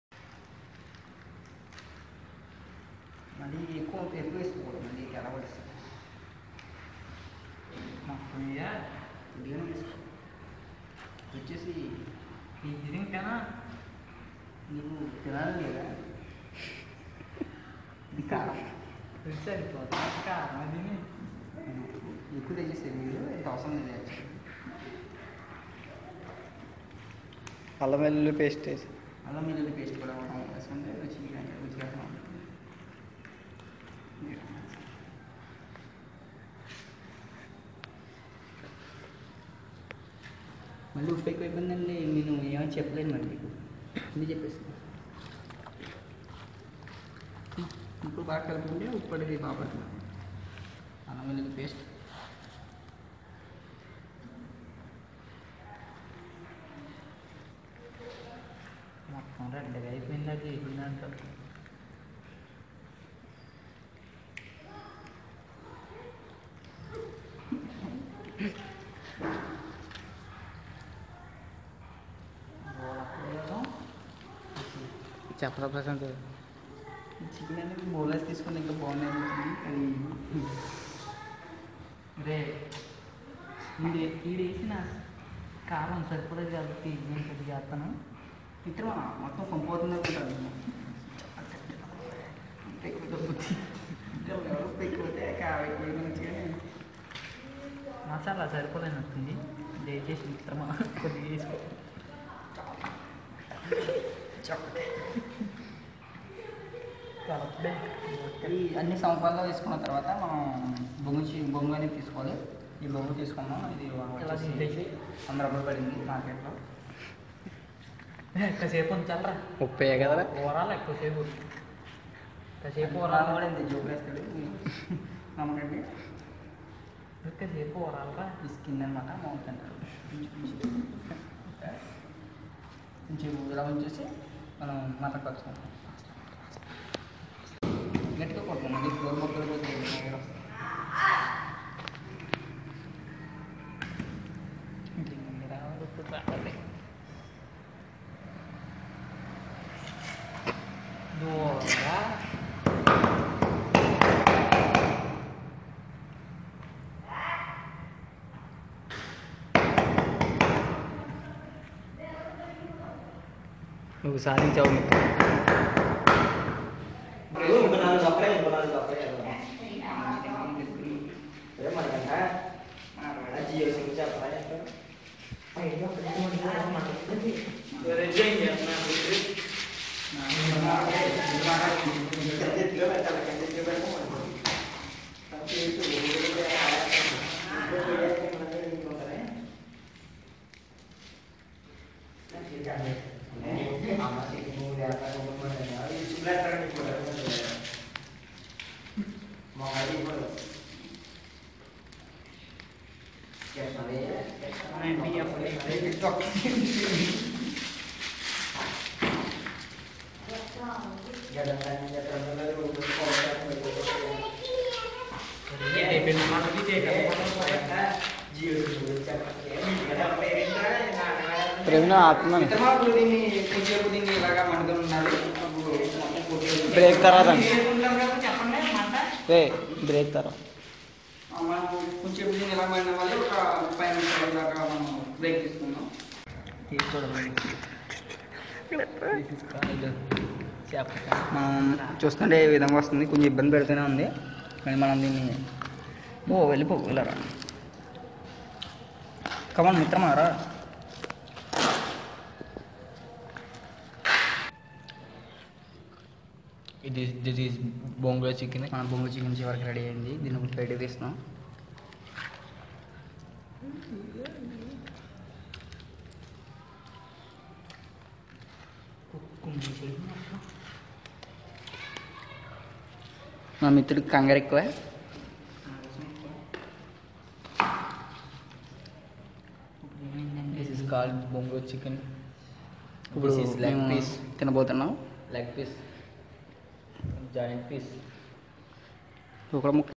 Bandarulanka bamboo chicken by vamsi chandana prasanth inumarthy and praveen konda
Preparation  of bomgu chicken